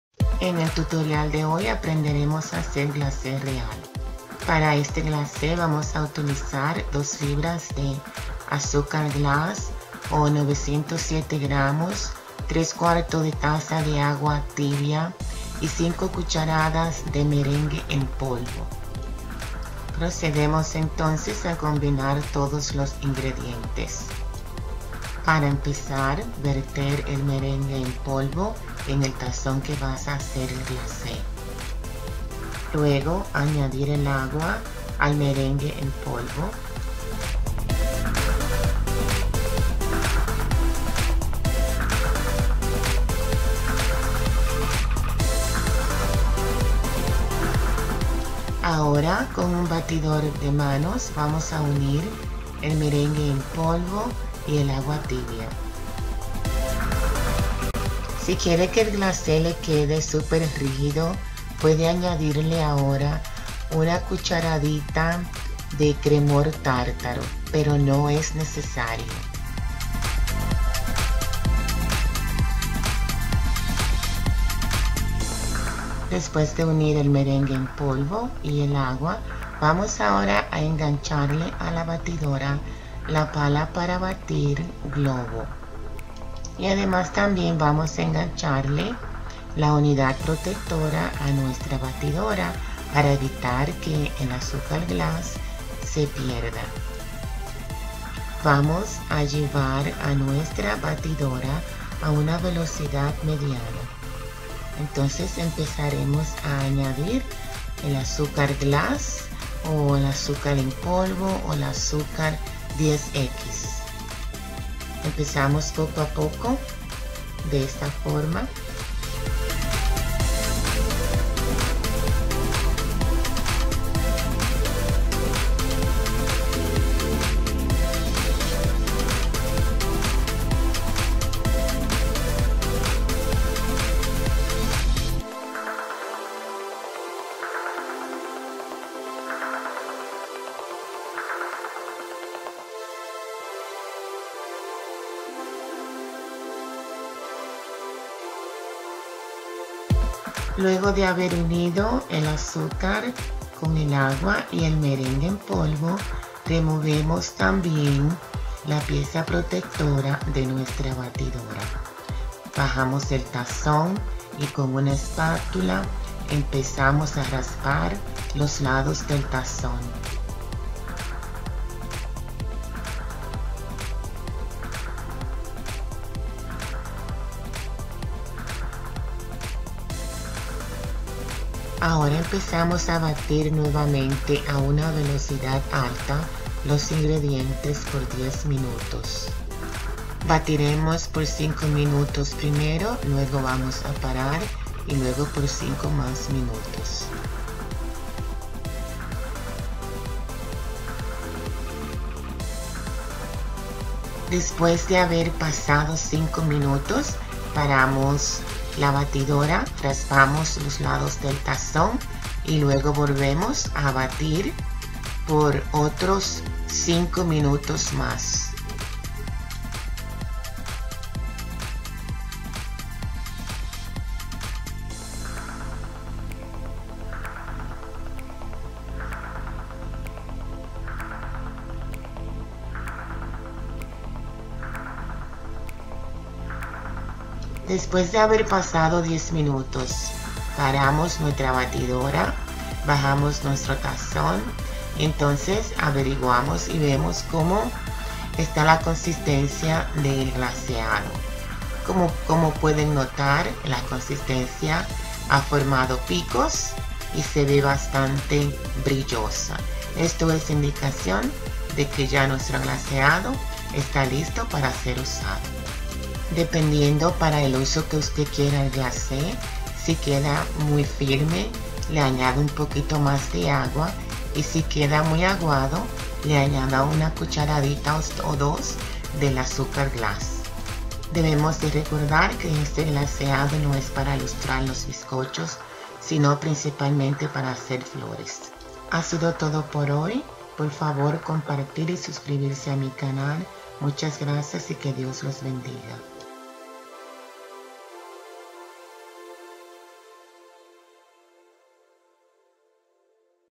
Como Hacer Glase Real
Este video te enseñara a como hacer glase real.

NECESITARÁS:

3/4 tazas de agua tibia
5 cdas. de merengue en polvo
1 cdta. de cremor tártaro (opcional)
2 lb. de azúcar en polvo o azúcar glas
Puedes también añadir 1 cucharadita de extractos como vainilla, etc.

"Cómo almacenar glaseado real
Guarda el glaseado real hecho con polvo de merengue en un recipiente hermético y mantenlo a temperatura ambiente hasta por 2 semanas. Antes de usar, batir bien en un vaso mezclador a baja velocidad. Si no tiene planes de usar el glaseado dentro de 2 semanas, otra opción es sacar las decoraciones y dejar que se endurezcan. ¡Las decoraciones de glaseado real endurecido se pueden almacenar en un recipiente NO hermético cubierto (como una caja de pastel) durante años! Son geniales para tener a mano para futuros pasteles."
Extraido del sitio de web de Wilton.com